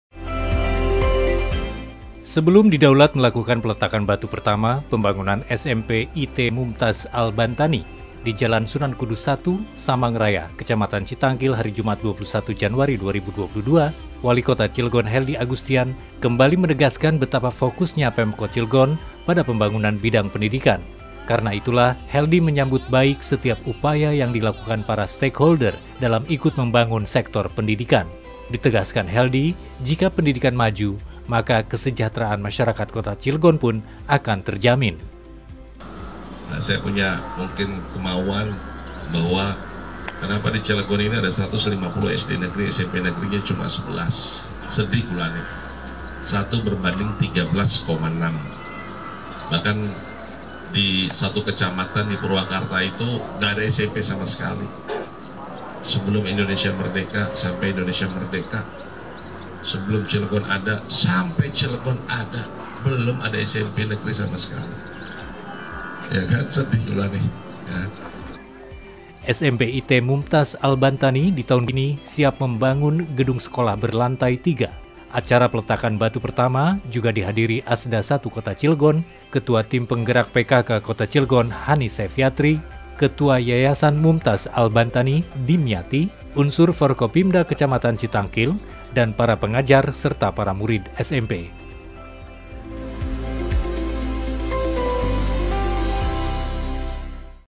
PELETAKAN BATU PERTAMA SMP IT MUMTAZ ALBANTANI OLEH WALI KOTA CILEGON
Sebelum didaulat melakukan peletakan batu pertama pembangunan SMP IT Mumtaz Al-Bantani, di JL Sunan Kudus I, Samangraya, Kecamatan Citangkil, Jumat 21 Januari 2022,  Wali Kota Cilegon Helldy Agustian kembali menegaskan betapa fokusnya Pemkot Cilegon pada pembangunan bidang pendidikan. Karena itulah Helldy menyambut baik setiap upaya yang dilakukan para stake holder dalam ikut membangun sektor pendidikan.